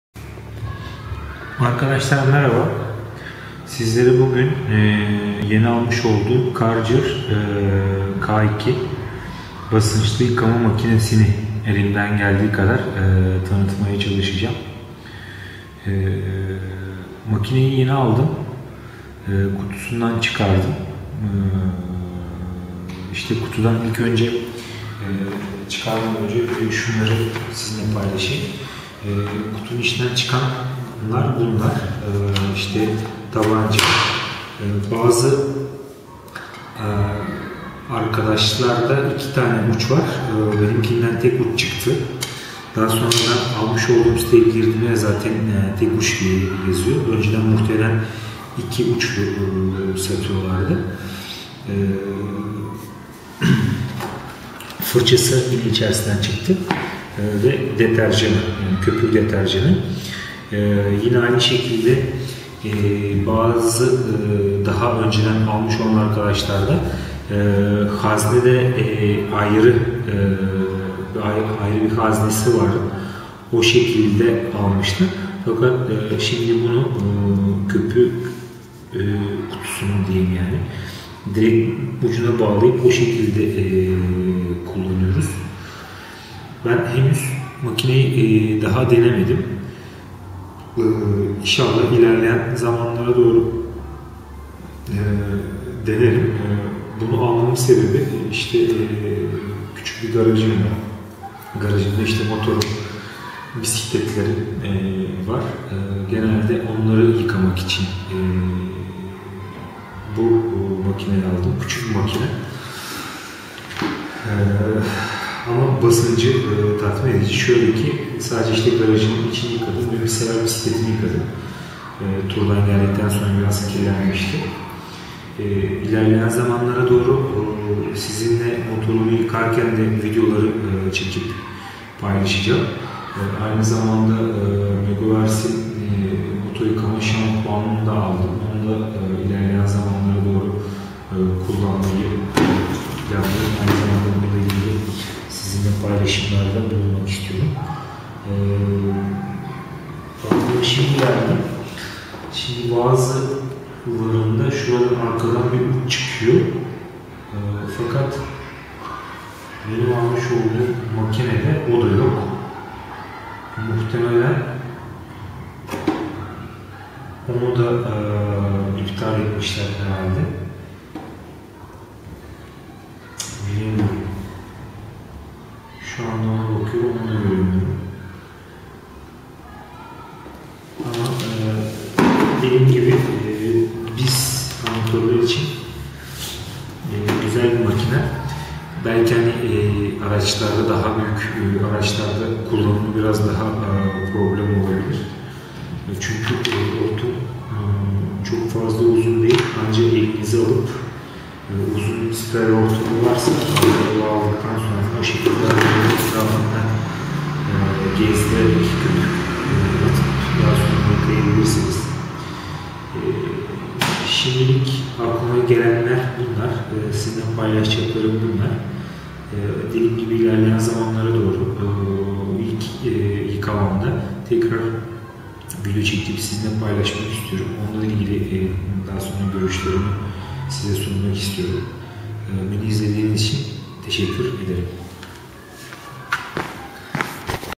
Karcher K2 Basınçlı Yıkama
Karcher K2 Öztürk Garajda Kutu Açılımı içinden çıkan parçaların tanıtım videosu.